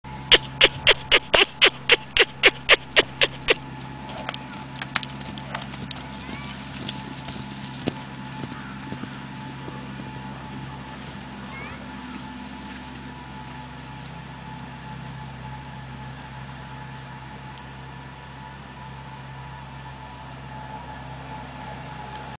Ace and Covbug in the pasture cont...
Cont.-Ace, (sorrel), and Covey, (grulla paint) having a ball turned out together for the first time.